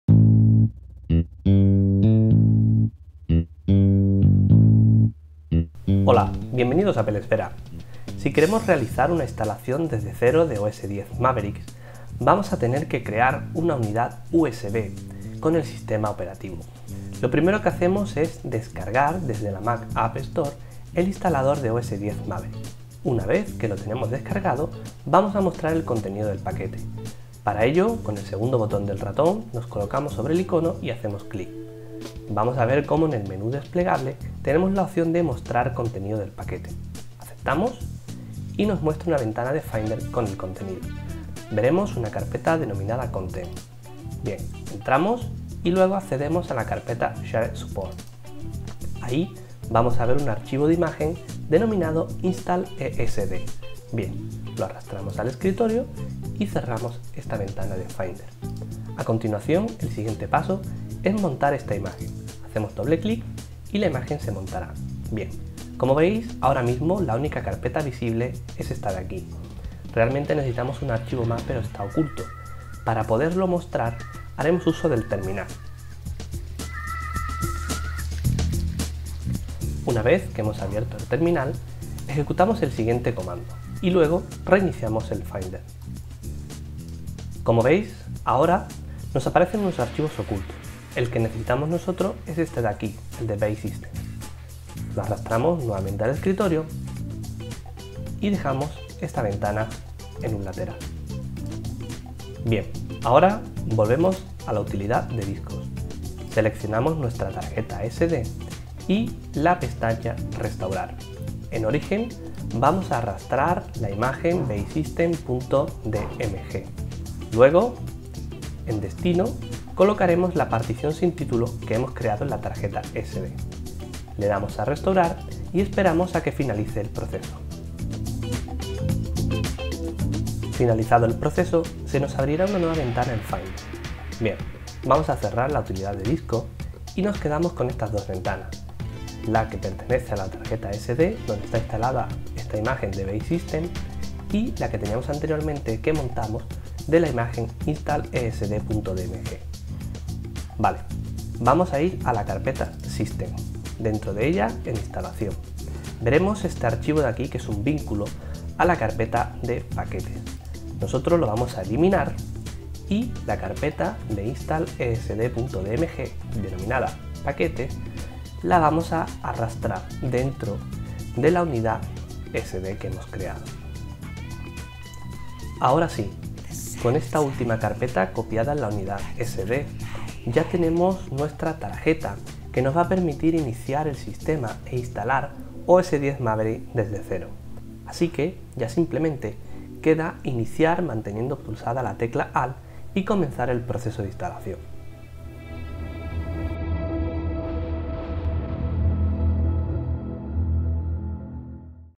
OS X Mavericks, cómo instalar desde cero
Tutorial para crear un instalar de OS X Mavericks en una unidad USB o tarjeta SD para realizar una instalación limpia o desde cero. Más detalles en Applesfera